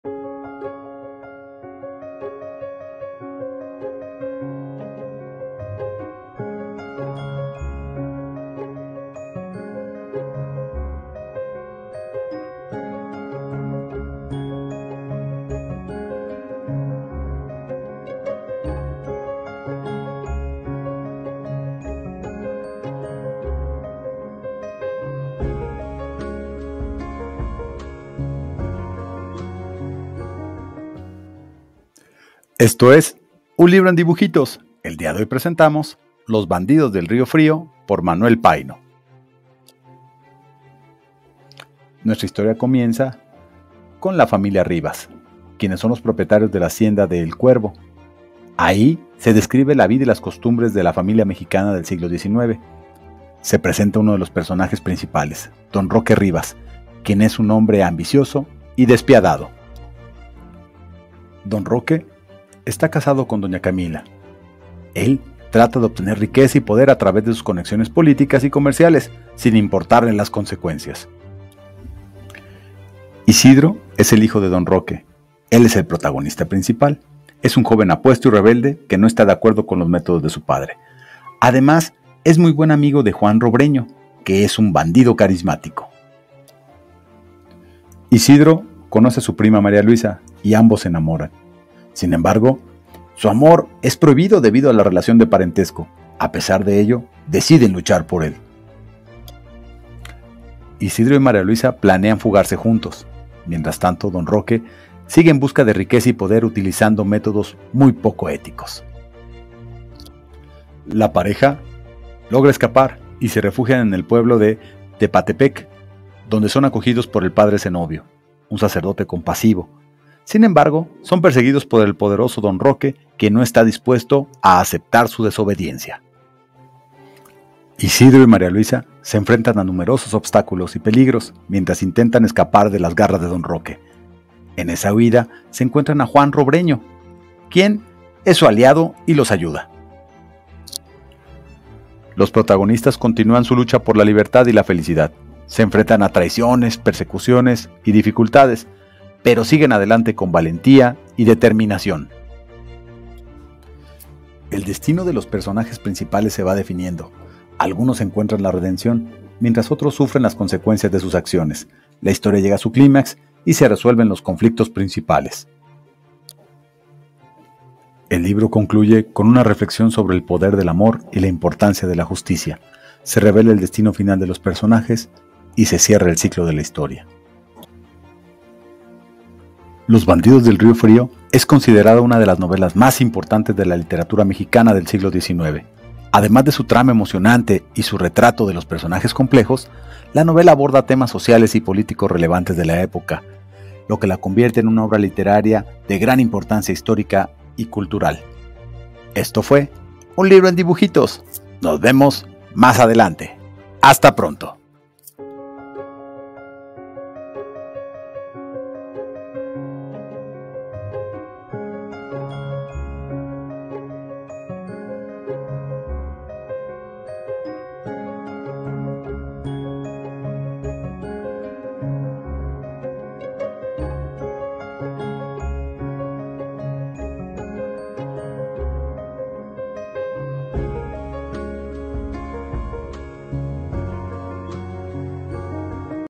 Los bandidos del Río Frío - Manuel Payno - Un Libro en Dibujitos (Resumen para Estudiantes)
Capítulo 1: El autor introduce a la familia de los Rivas, quienes son los propietarios de la hacienda de "El Cuervo". Se describe la vida y las costumbres de la alta sociedad mexicana del siglo XIX. Además, se presenta a uno de los personajes principales, Don Roque Rivas, quien es un hombre ambicioso y despiadado.

Capítulo 2: Se narra la historia de Don Roque Rivas y su matrimonio con Doña Camila. Don Roque trata de obtener riqueza y poder a través de sus conexiones políticas y comerciales, sin importarle las consecuencias.

Capítulo 3: Se presenta a Isidro, el hijo de Don Roque, quien es el protagonista principal de la historia. Isidro es un joven apuesto y rebelde que no está de acuerdo con los métodos de su padre. También se introduce a otros personajes secundarios, como Juan Robreño, un bandido carismático.

Capítulo 4: Isidro conoce a su prima María Luisa, y ambos se enamoran. Sin embargo, su amor es prohibido debido a la relación de parentesco. A pesar de ello, deciden luchar por su amor.

Capítulo 5: Isidro y María Luisa planean fugarse juntos para poder estar juntos. Mientras tanto, Don Roque sigue en busca de riqueza y poder, utilizando métodos poco éticos.

Capítulo 6: Isidro y María Luisa logran escapar y se refugian en el pueblo de Tepatepec, donde son acogidos por el Padre Cenobio, un sacerdote compasivo. Sin embargo, son perseguidos por el poderoso Don Roque, quien no está dispuesto a aceptar su desobediencia.

Capítulo 7: Isidro y María Luisa se enfrentan a numerosos obstáculos y peligros mientras intentan escapar de las garras de Don Roque. Durante su huida, encuentran a Juan Robreño, el bandido, quien se convierte en su aliado.

Capítulo 8: Los protagonistas continúan su lucha por la libertad y la felicidad. Se enfrentan a traiciones, persecuciones y dificultades, pero siguen adelante con valentía y determinación.

Capítulo 9: El destino de los personajes principales se va definiendo. Algunos encuentran la redención, mientras que otros sufren las consecuencias de sus acciones. La historia llega a su clímax y se resuelven los conflictos principales.

Capítulo 10: El libro concluye con una reflexión sobre el poder del amor y la importancia de la justicia. Se revela el destino final de los personajes y se cierra el ciclo de la historia.